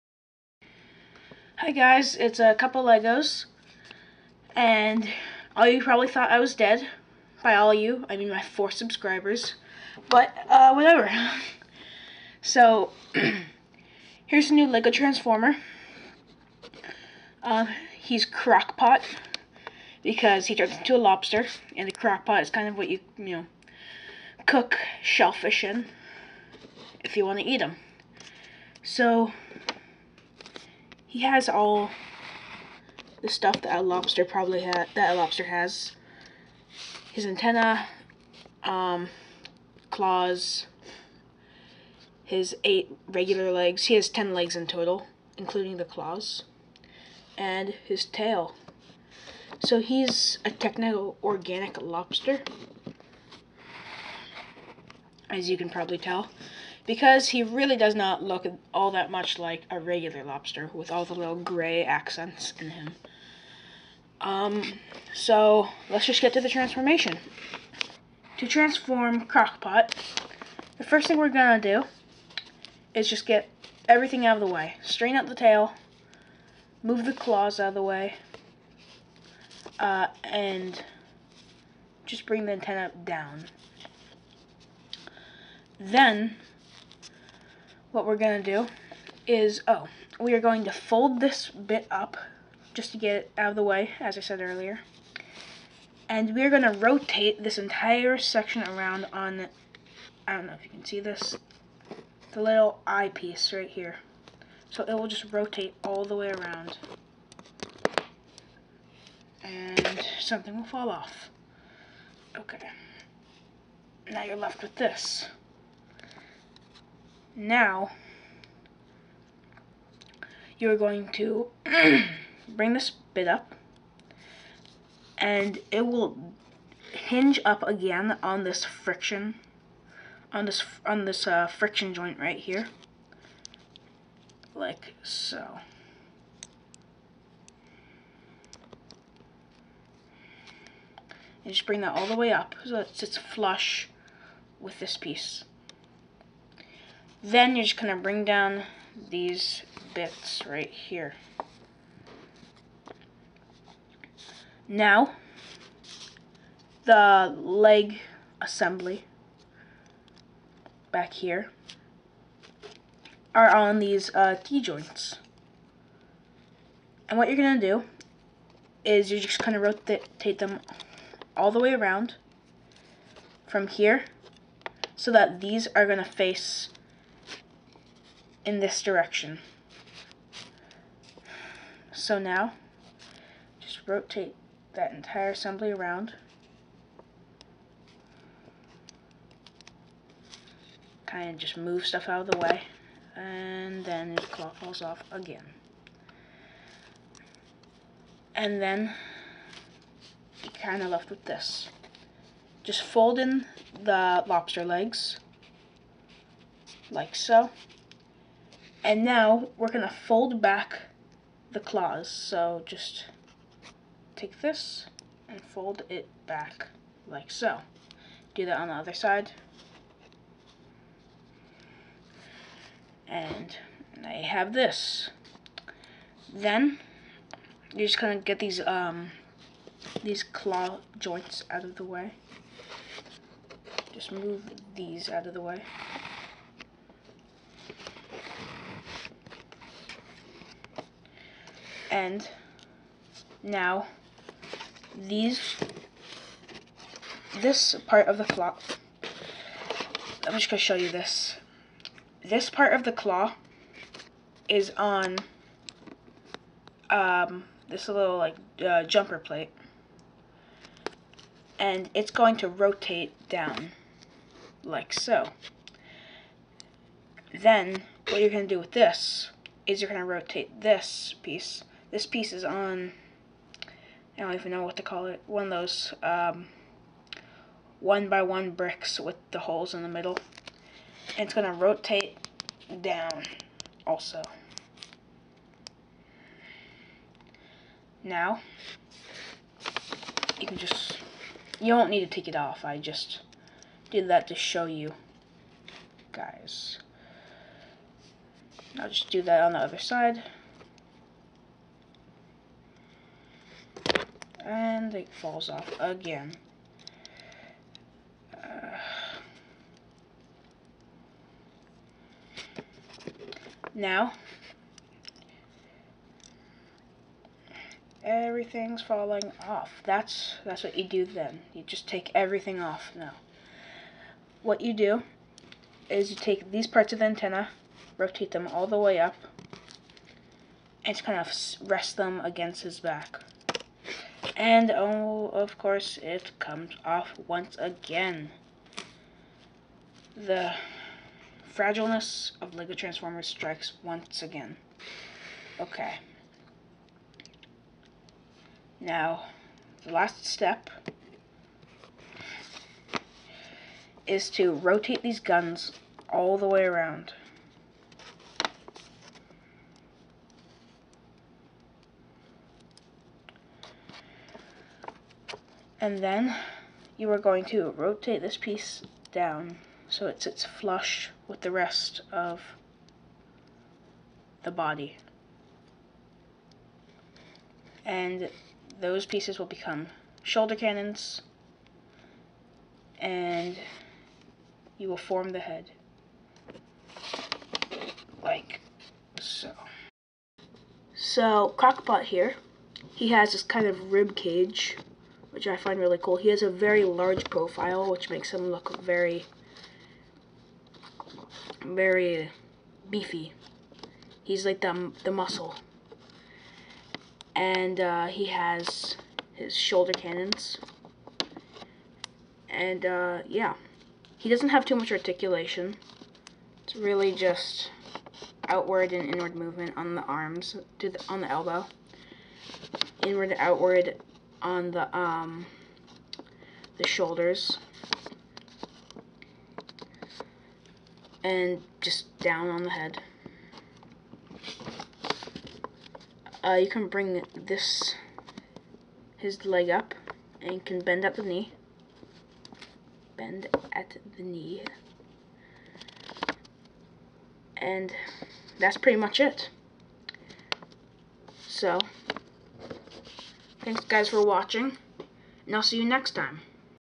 Lego Transformers #4 Crock Pot
Crock Pot is a autobot warrior with little brains.  In fact, being an arthropod, he has no brain at all!  However, his brute force in the autobot team makes up for his lack of intelligence.